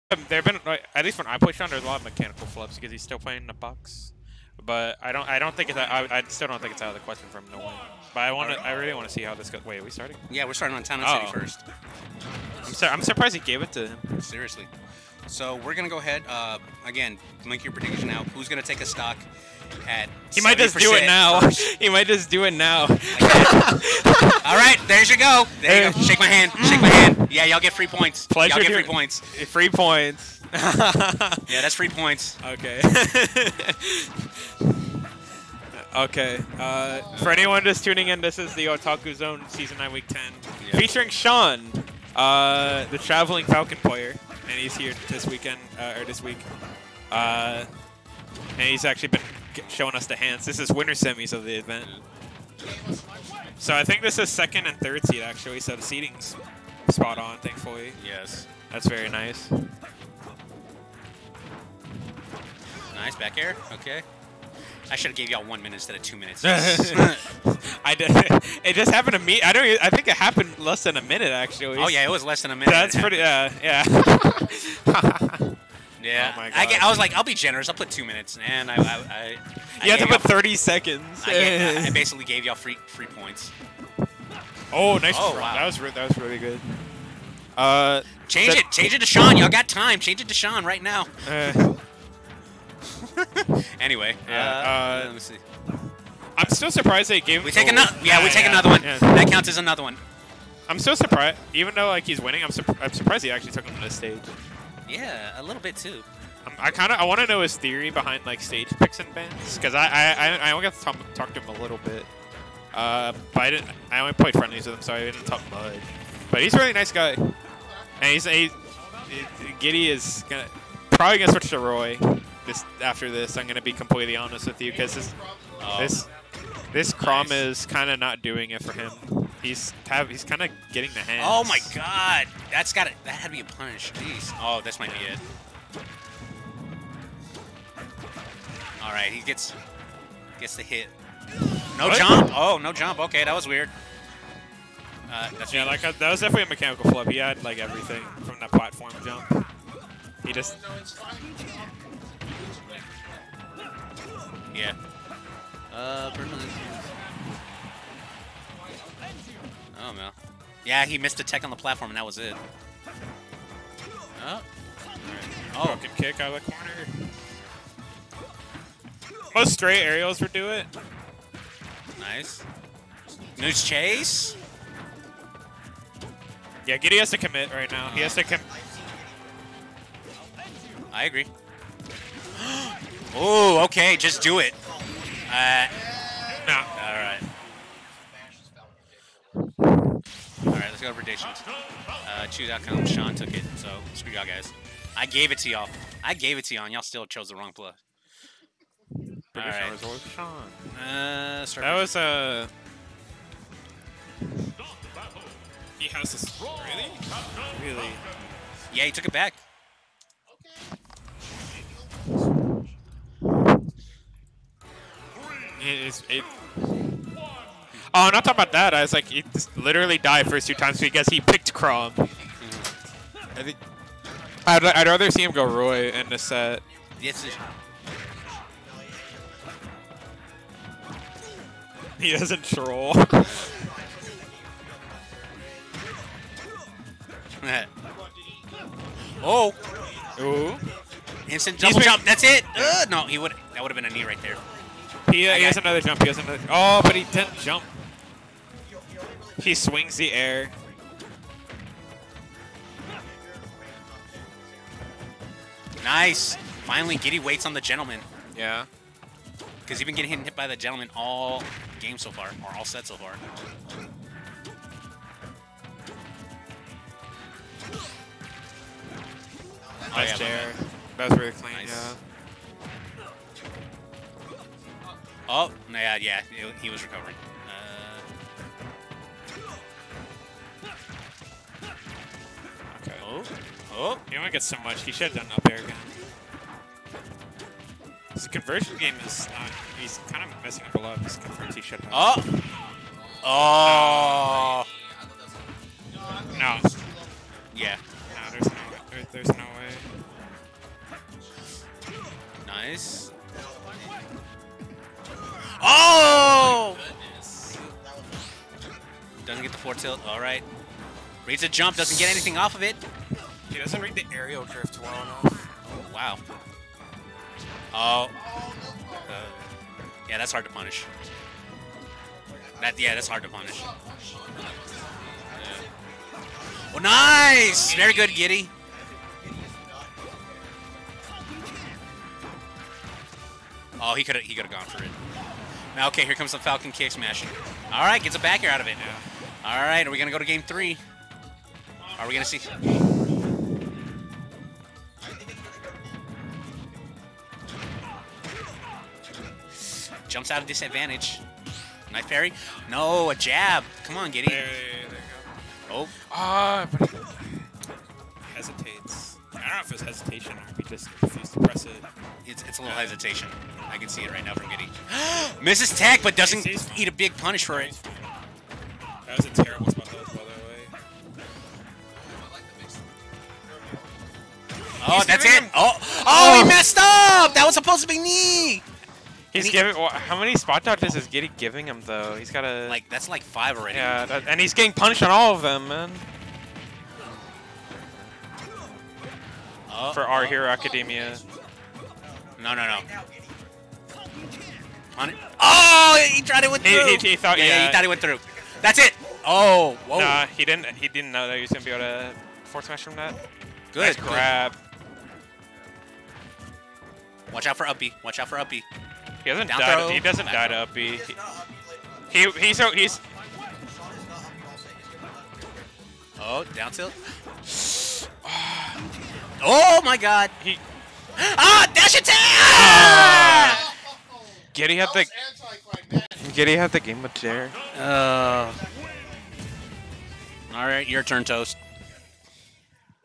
OZone9W10 - WS - Sean vs Gidy
57 Entrants
Winners Semis
Sean (P1 C. Falcon) vs Gidy (P2 Chrom)
2:50:24 - 2:57:37